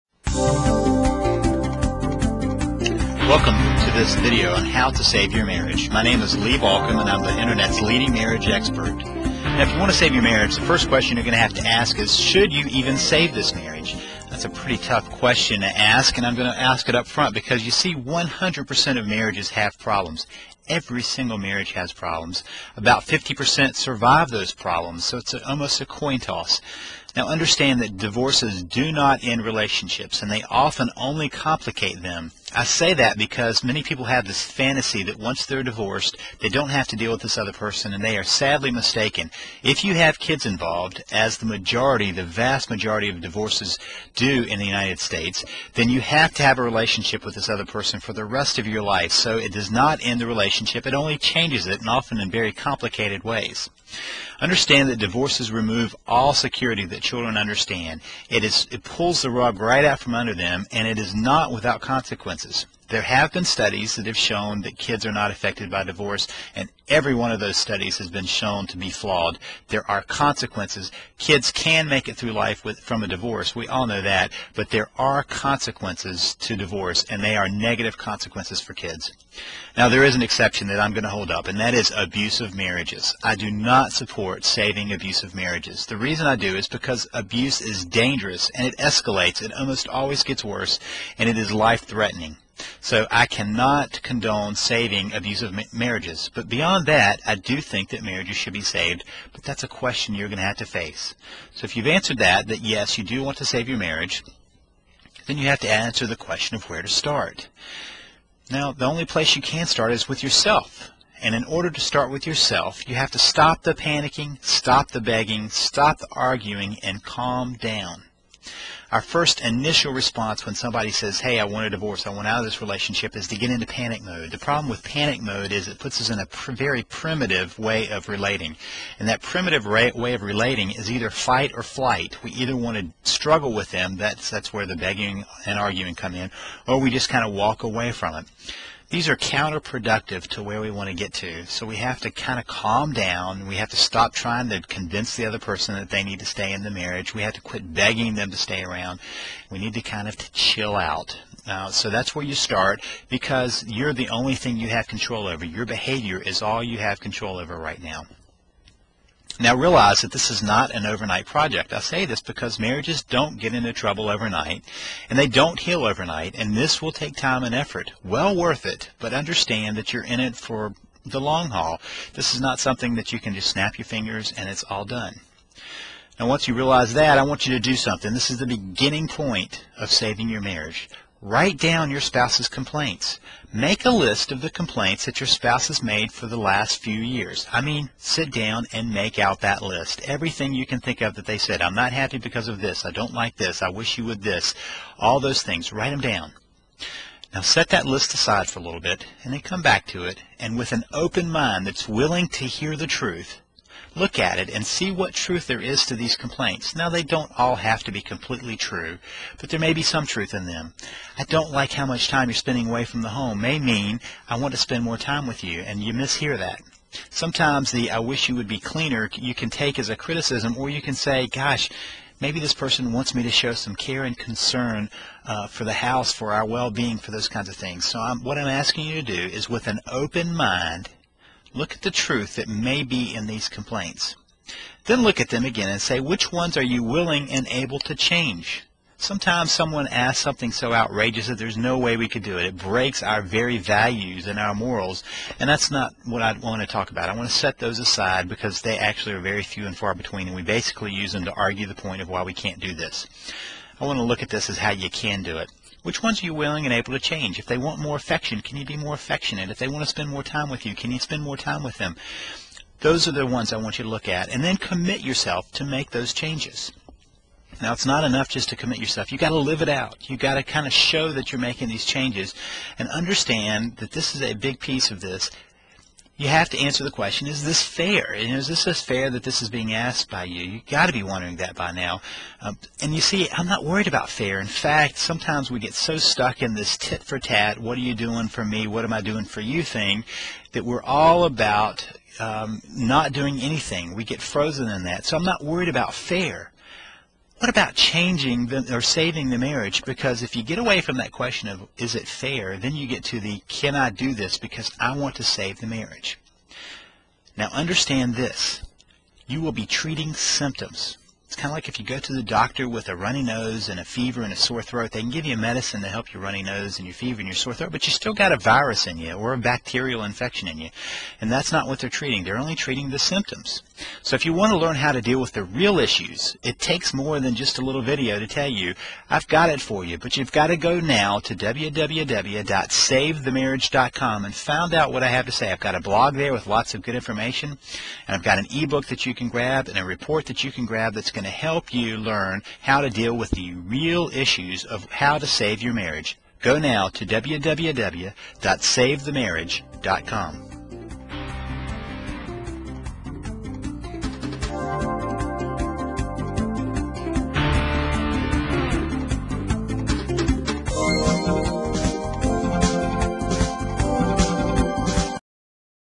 How To Save Your Marriage
You can start saving your marriage now, if you know how.  Save a marriage by taking these first steps.  How To Save A Marriage gives you a starting point.